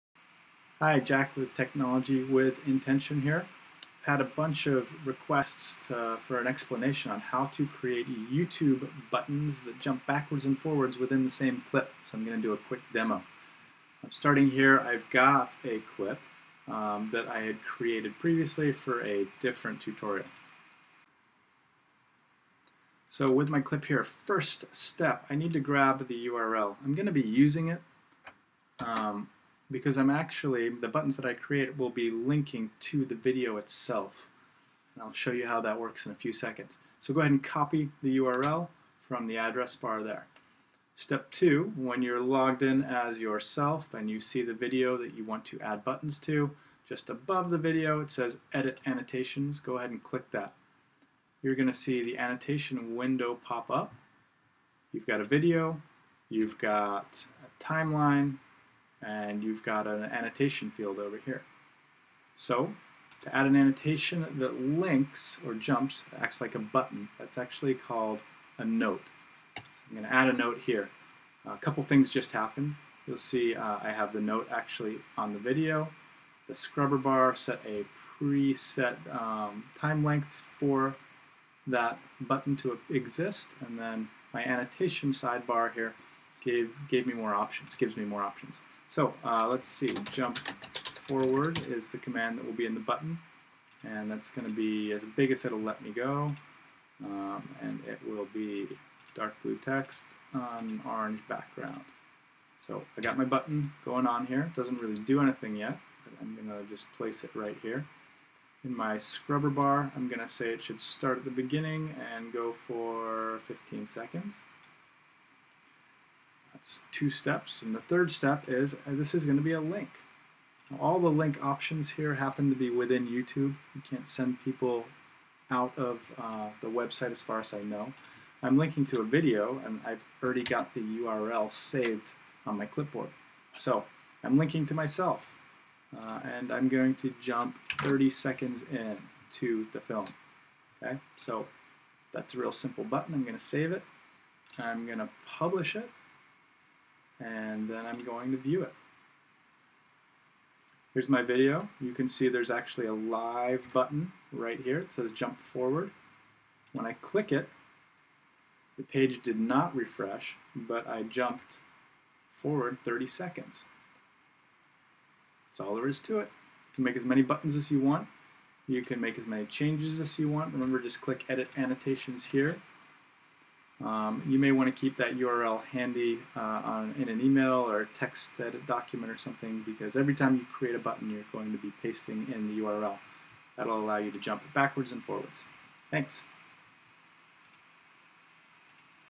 Jump backwards & forwards within a YouTube video
Jump backwards & forwards within a YouTube video. Using annotations to create interactive flipped instruction lessons.